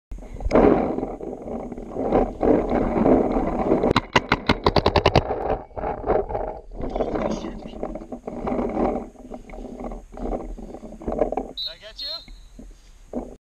GoPro split screen
short split screen video with some run and gun action.